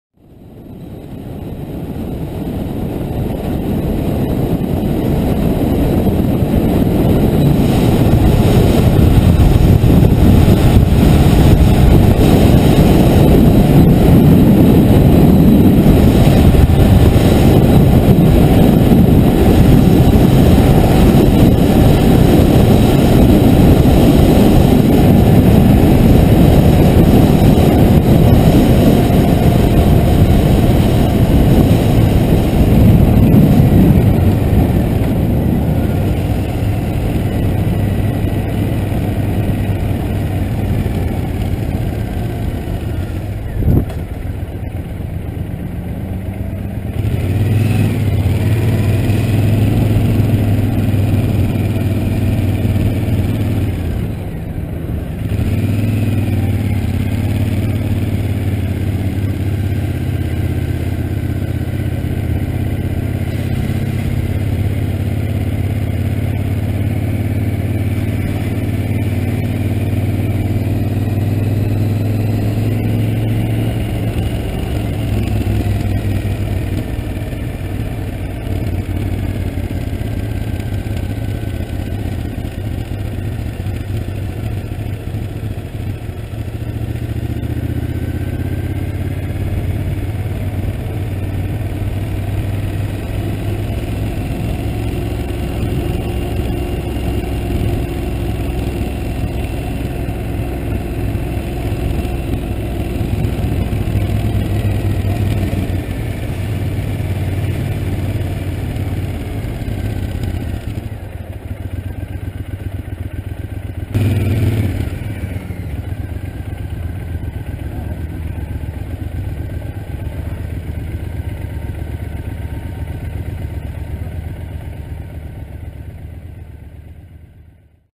FortRoseCrans2016
Motorcycle ride to Fort Rosecrans point loma san diego